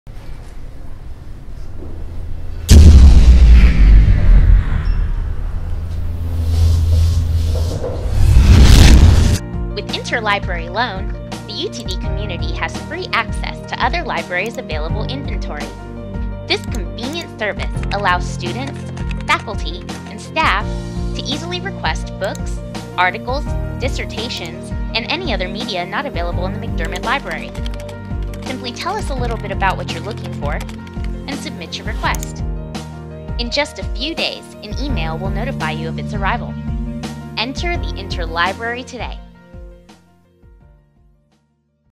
Interlibrary Loans at UT Dallas McDermott Library
Interlibrary loan is a valuable service to provide material to students, even if McDermott library does not own it!